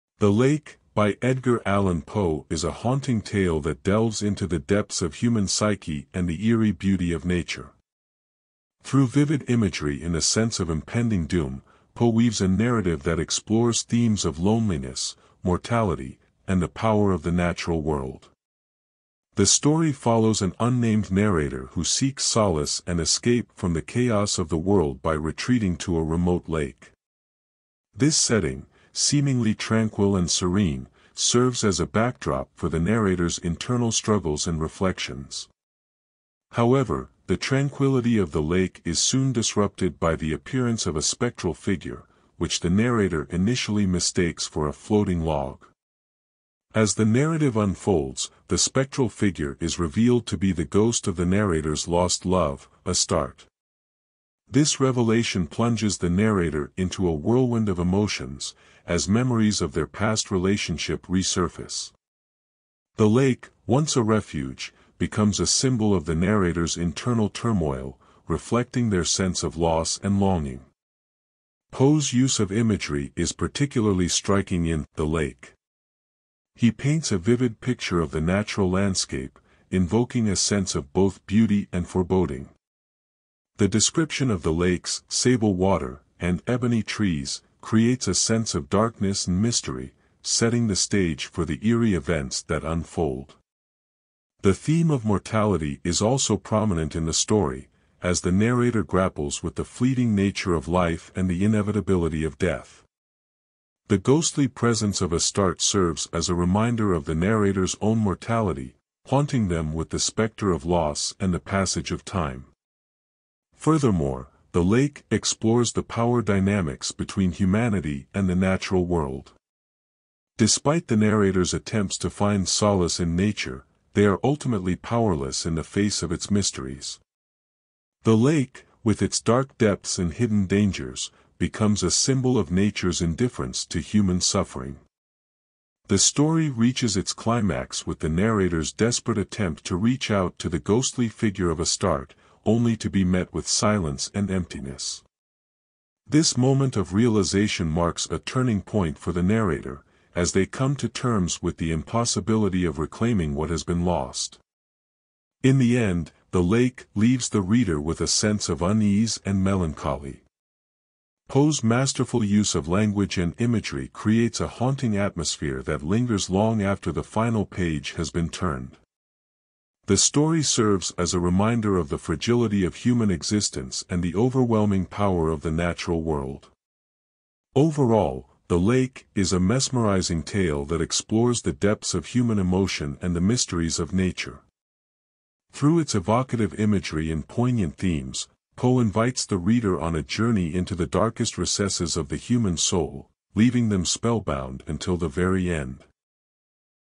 "The Lake" By Edgar Allan Poe
"The Lake" by Edgar Allan Poe is a haunting tale that delves into the depths of human psyche and the eerie beauty of nature. Through vivid imagery and a sense of impending doom, Poe weaves a narrative that explores themes of loneliness, mortality, and the power of the natural world.
The story follows an unnamed narrator who seeks solace and escape from the chaos of the world by retreating to a remote lake. This setting, seemingly tranquil and serene, serves as a backdrop for the narrator's internal struggles and reflections. However, the tranquility of the lake is soon disrupted by the appearance of a spectral figure, which the narrator initially mistakes for a floating log.
As the narrative unfolds, the spectral figure is revealed to be the ghost of the narrator's lost love, Astarte. This revelation plunges the narrator into a whirlwind of emotions, as memories of their past relationship resurface. The lake, once a refuge, becomes a symbol of the narrator's internal turmoil, reflecting their sense of loss and longing.
Poe's use of imagery is particularly striking in "The Lake." He paints a vivid picture of the natural landscape, invoking a sense of both beauty and foreboding. The description of the lake's "sable water" and "ebony trees" creates a sense of darkness and mystery, setting the stage for the eerie events that unfold.
The theme of mortality is also prominent in the story, as the narrator grapples with the fleeting nature of life and the inevitability of death. The ghostly presence of Astarte serves as a reminder of the narrator's own mortality, haunting them with the specter of loss and the passage of time.
Furthermore, "The Lake" explores the power dynamics between humanity and the natural world. Despite the narrator's attempts to find solace in nature, they are ultimately powerless in the face of its mysteries. The lake, with its dark depths and hidden dangers, becomes a symbol of nature's indifference to human suffering.
The story reaches its climax with the narrator's desperate attempt to reach out to the ghostly figure of Astarte, only to be met with silence and emptiness. This moment of realization marks a turning point for the narrator, as they come to terms with the impossibility of reclaiming what has been lost.
In the end, "The Lake" leaves the reader with a sense of unease and melancholy. Poe's masterful use of language and imagery creates a haunting atmosphere that lingers long after the final page has been turned. The story serves as a reminder of the fragility of human existence and the overwhelming power of the natural world.
Overall, "The Lake" is a mesmerizing tale that explores the depths of human emotion and the mysteries of nature. Through its evocative imagery and poignant themes, Poe invites the reader on a journey into the darkest recesses of the human soul, leaving them spellbound until the very end.